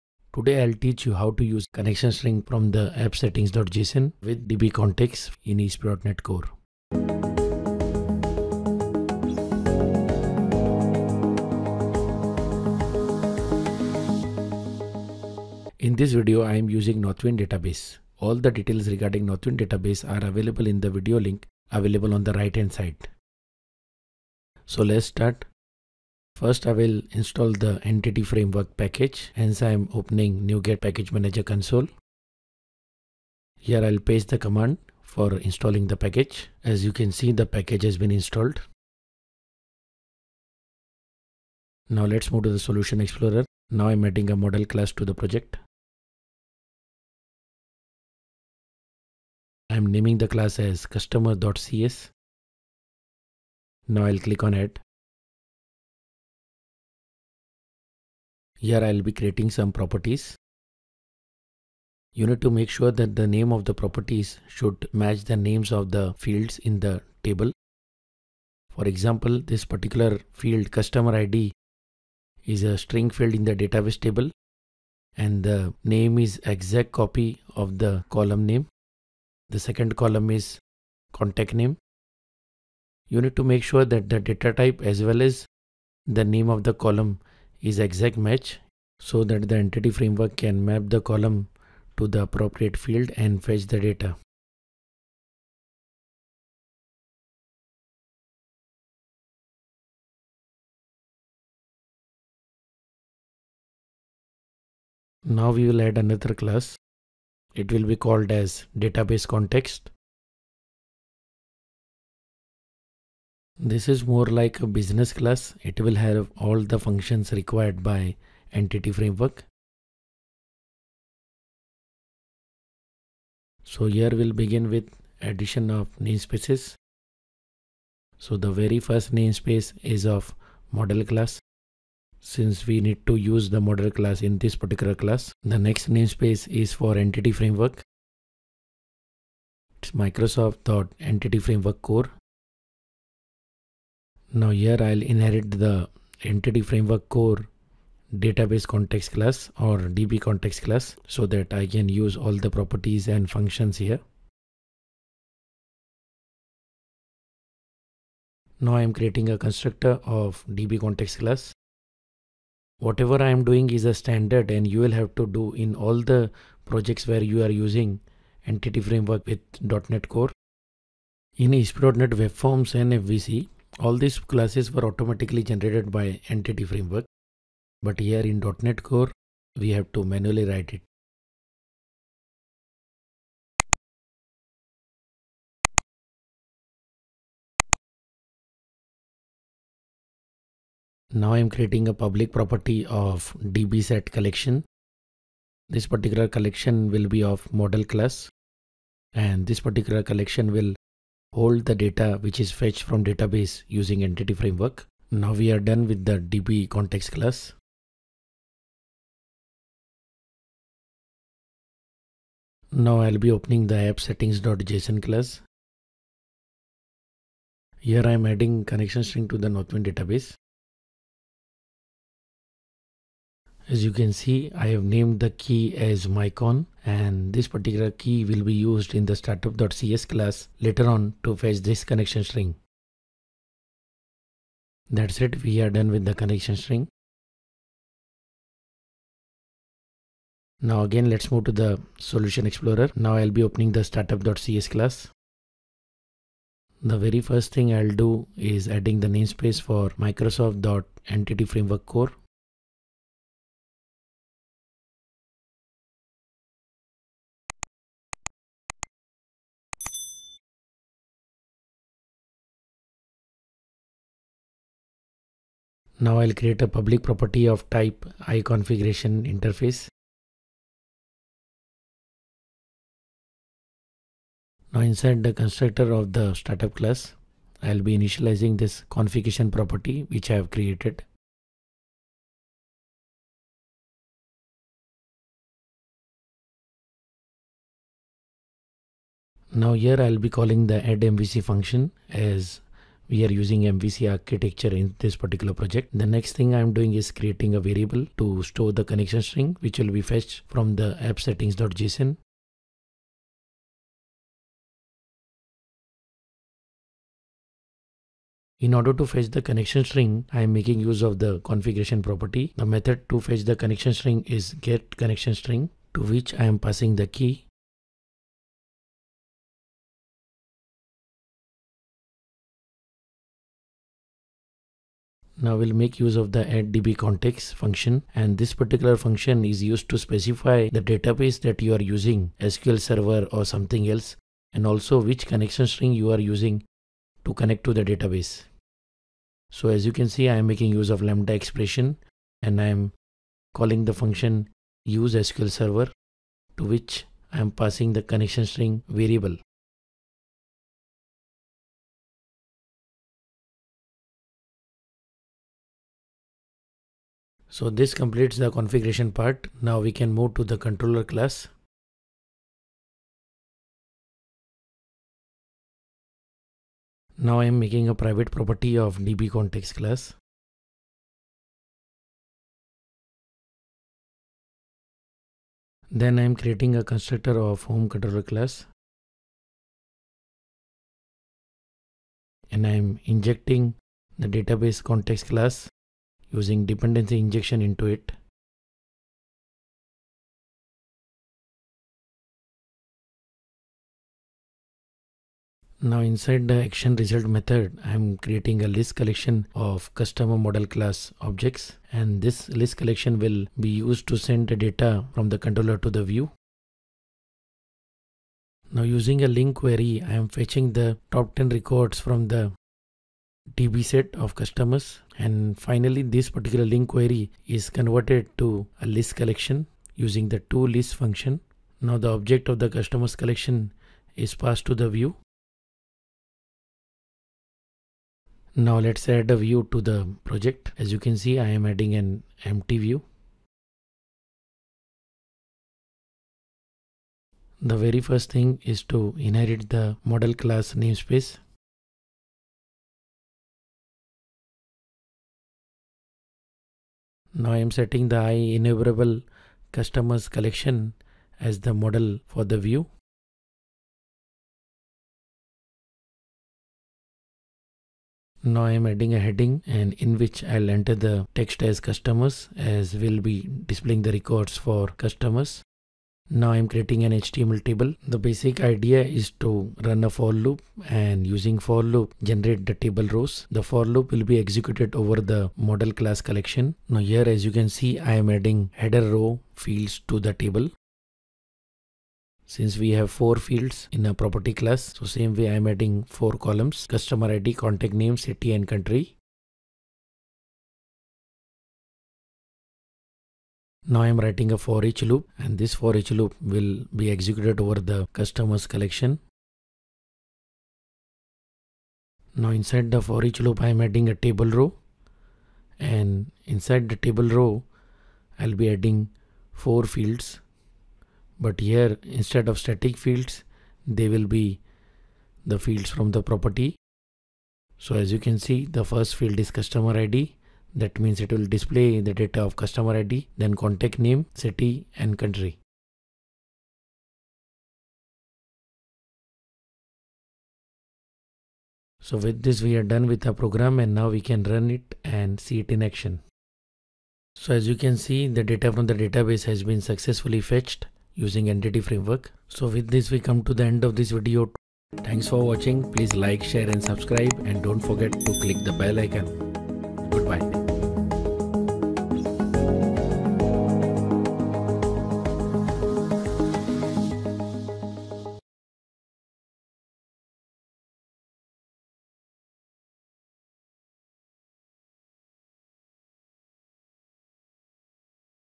Use Connection String from AppSettings.json with DbContext in ASP.Net Core
Here Mudassar Khan has explained how to set use Connection String from AppSettings.json with DbContext class in .Net Core.

Inside the Startup class, the Connection String is read from the AppSettings.json file and is used to add the DbContext service.

Chapters :
00:00 Introduction
00:15 Database Information
00:30 Installing Entity Framework Core package
00:52 Adding Model Class
01:57 Configuring Database Context
03:34 Adding Connection string in AppSettings.json
04:09 Configuring connection to the Database
06:34 Fetching the data from Database
07:45 Displaying data in HTML Table
08:56 Demo